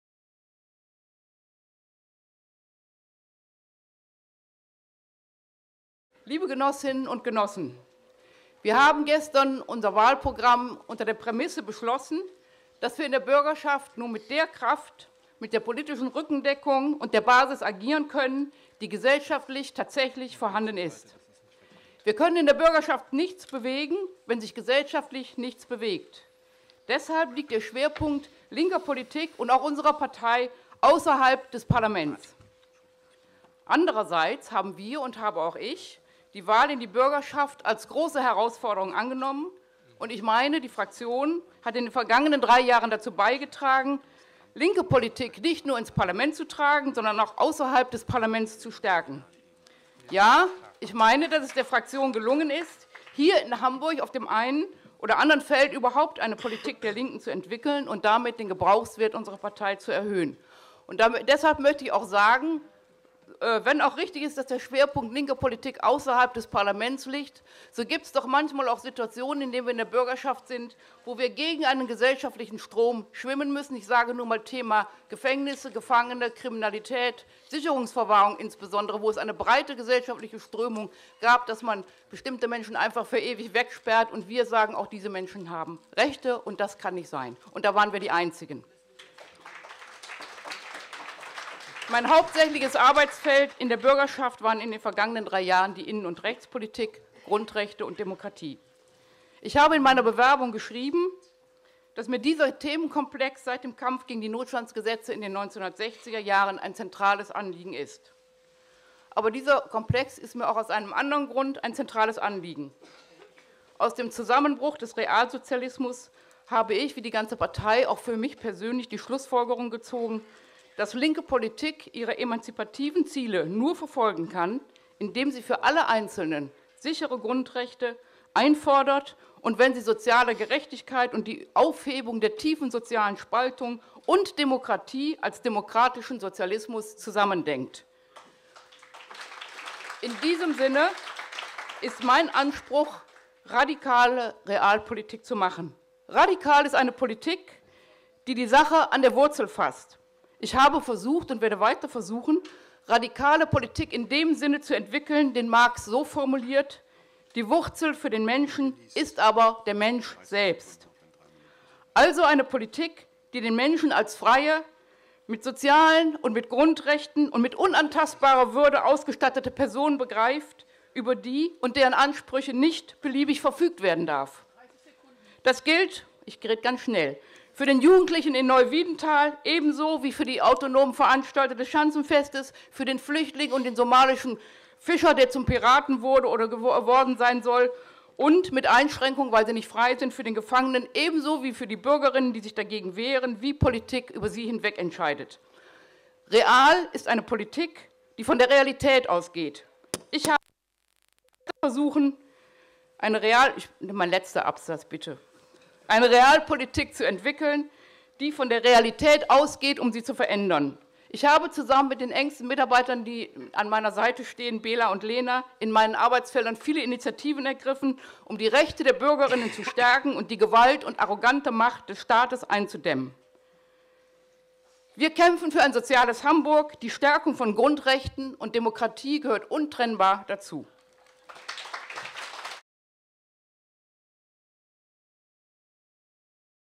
"Radikale Realpolitik für Hamburg"
Vorstellungsrede von Christiane Schneider, Kandidatin auf Platz 3 der Landesliste der LINKEN zur Bürgerschaftswahl in Hamburg, auf dem Landesparteitag am 9. Januar 2011